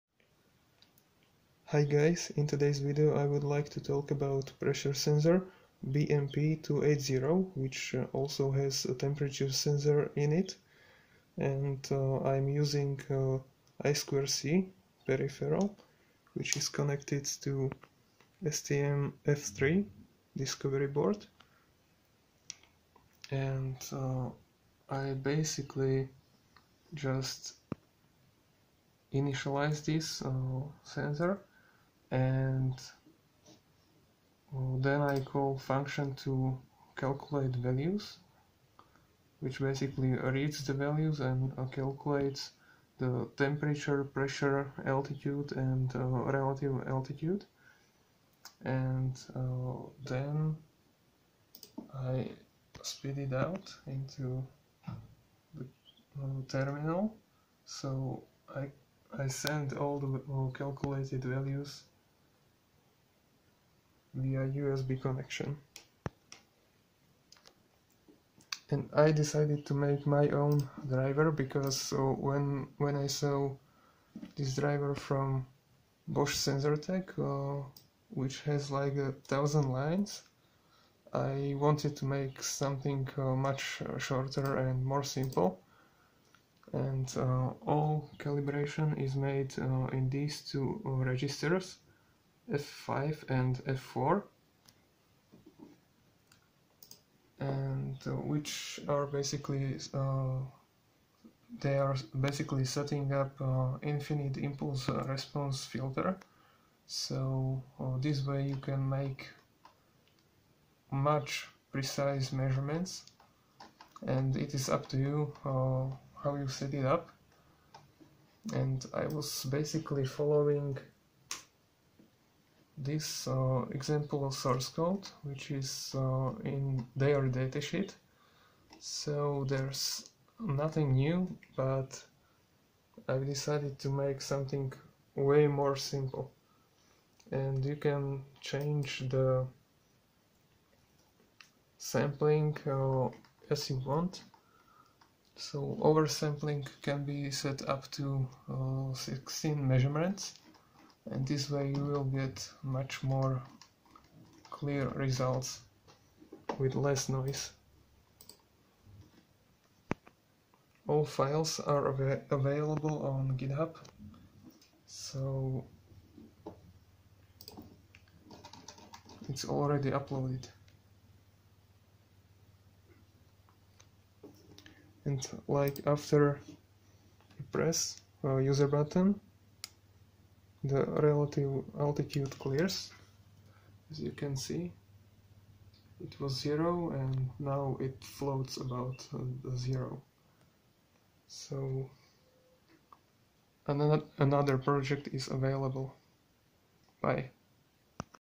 A simple driver for BMP280 using I2C - STM32 F3 Discovery
I have made my own driver for a pressure sensor BMP280.
It runs on STM32 F3 Discovery board.